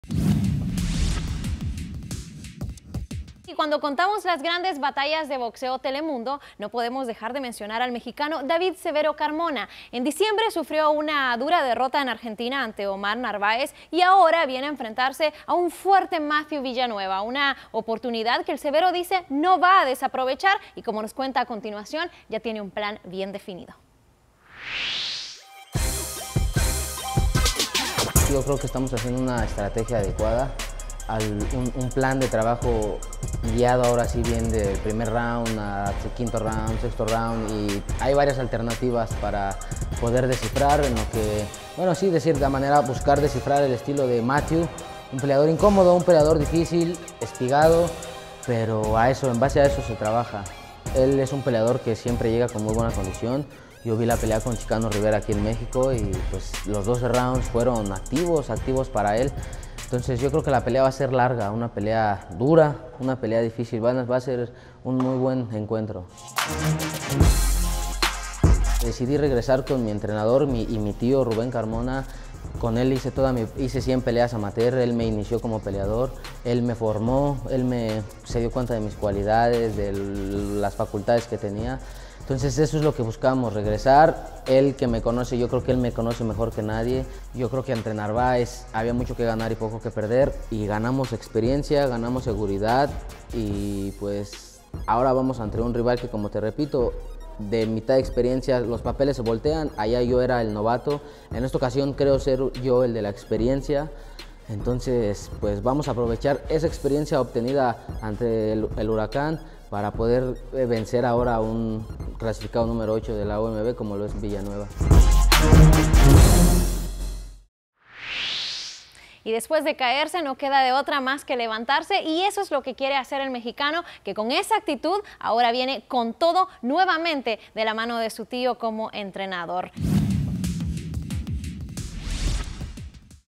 David Carmona vs Matthew Villanueva Previa | Boxeo | NBC Deportes
Video Oficial de Telemundo NBC Deportes.  El boxeador mexicano, David Carmona, ha estado entrenando con su tío y estudiando a su oponente para reecontrarse con la victoria en su próxima pelea frente a Matthew Villanueva.